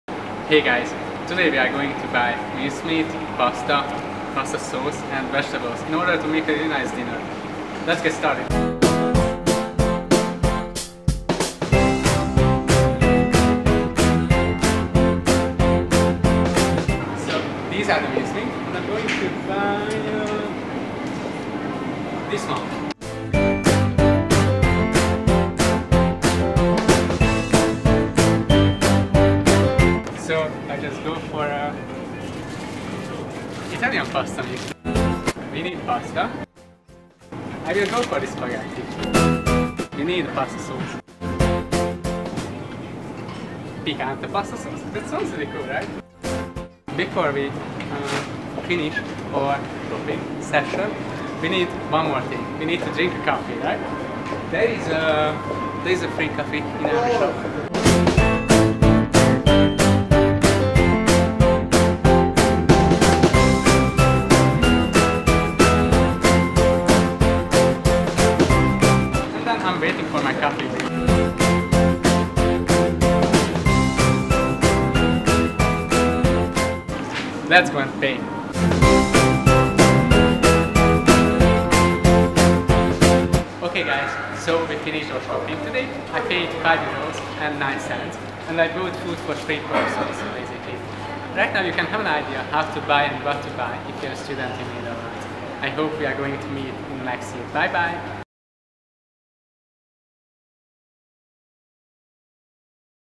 Supermarket tour In the Netherlands
🧐 Wondering how much you're going to spend while living in the Netherlands? 💸

Roland takes you on a supermarket tour.
He spends €5 to make pasta for 4 people! 🍝

👉 Do you have any questions for Roland and other international students?

Feeling social?